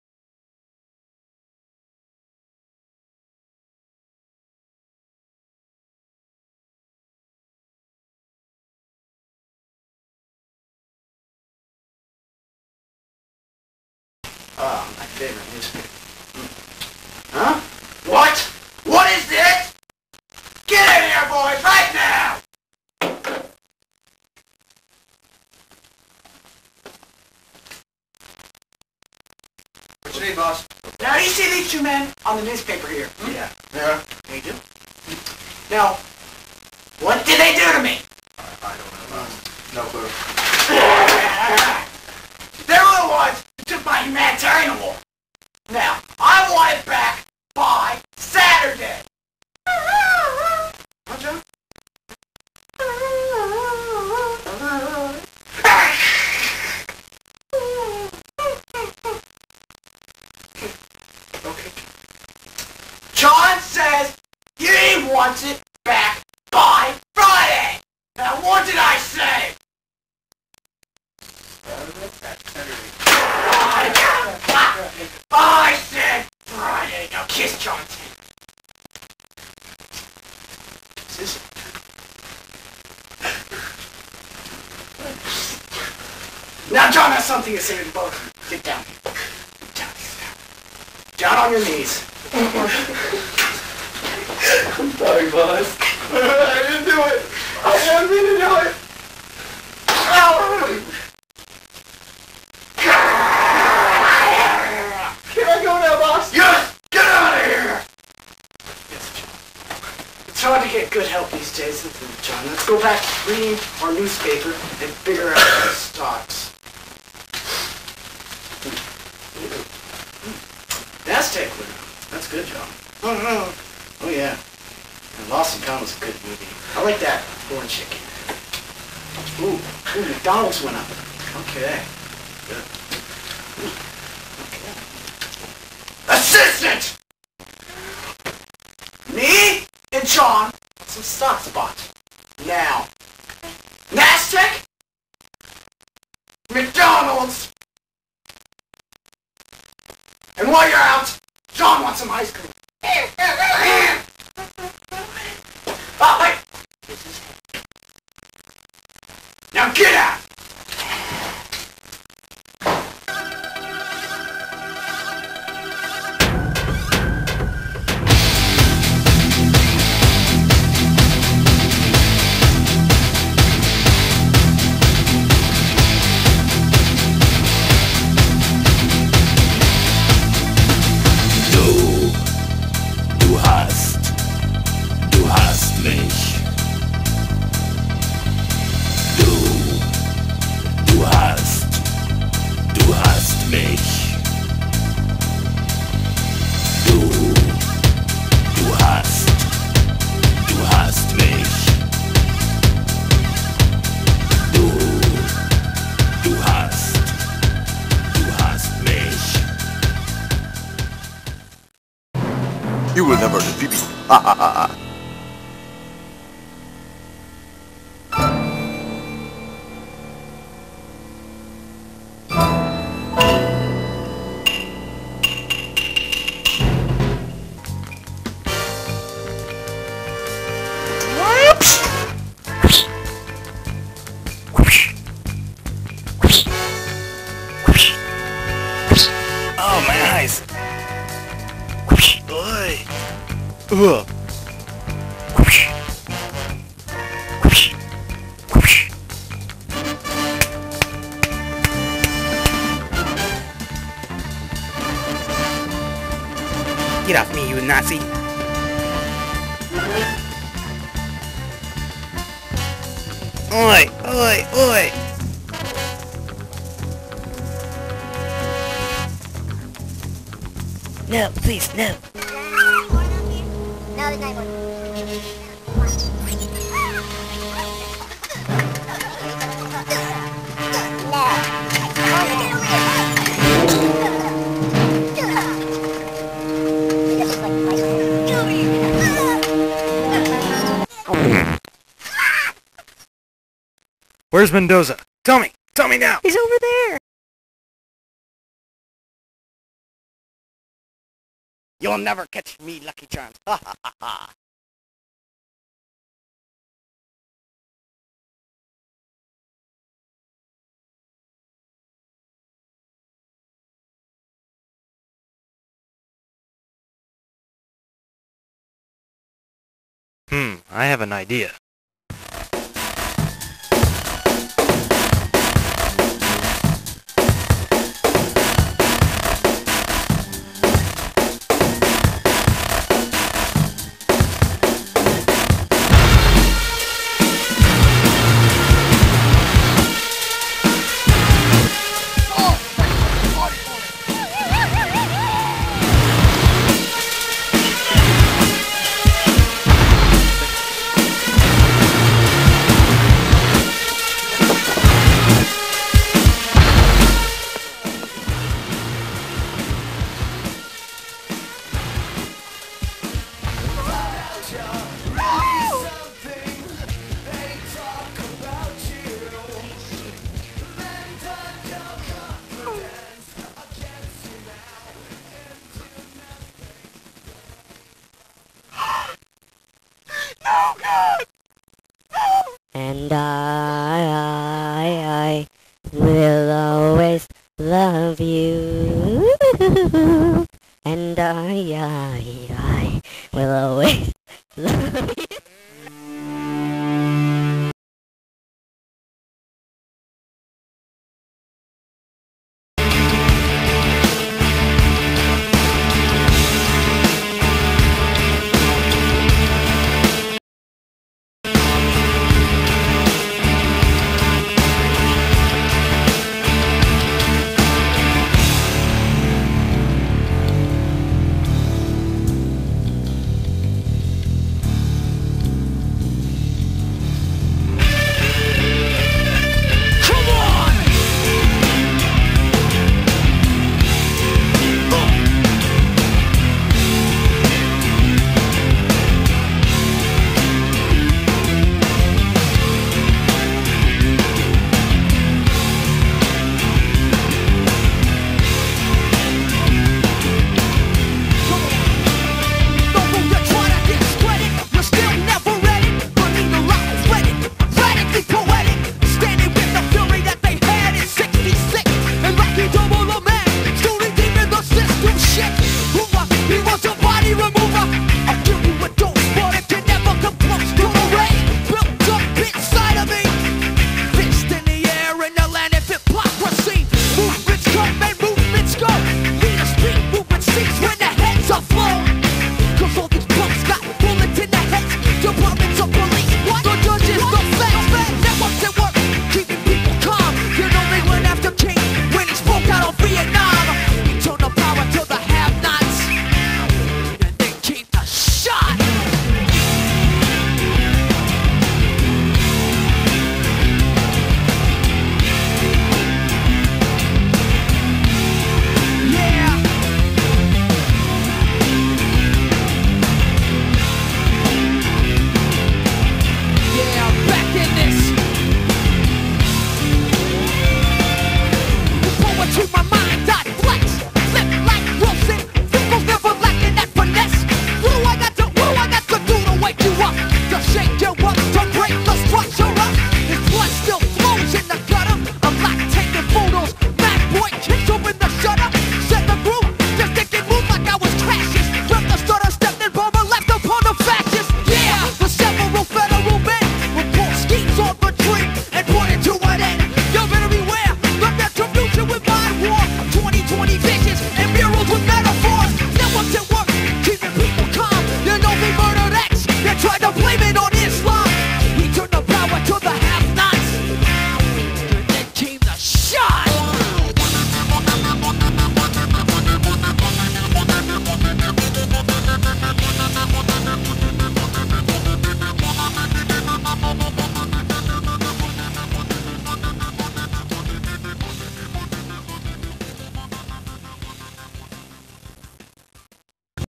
Hydroplane
Our story begins with two escaped convicts, Cyrus Grissom and Bruce Wayne.  Awhile back as a practical joke, the two stole an evil mob boss's Humanitarian Award.  Then the two were captured by the police for a previous theft.  Now that they have escaped the mob boss, Don Mendoza wants to get his revenge.